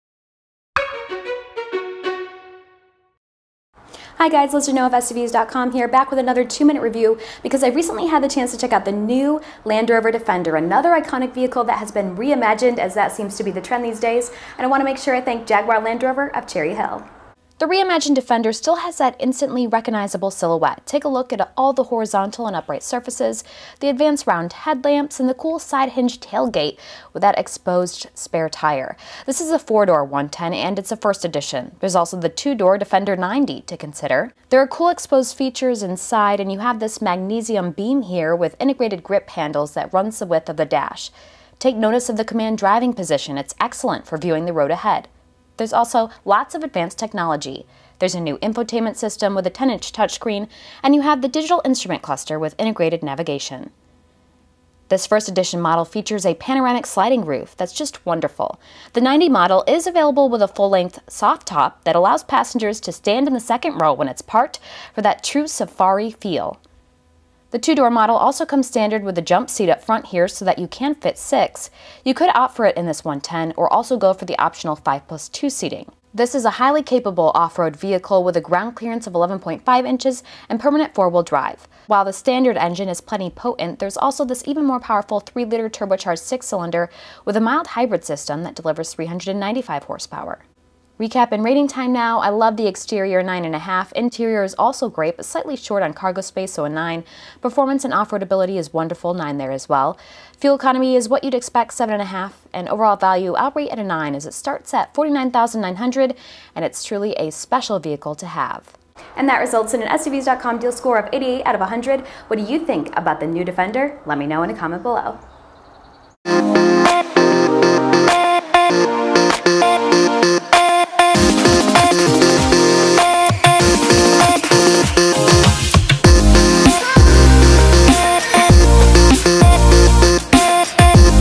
2020 Land Rover Defender - 2 Minute Review - SUVS.com
The Land Rover Defender is back! Watch as Liz takes a look at the four-door Defender, which has been reimagined for the 21st century.

Looking for a new SUV? Compare and shop the best deals near you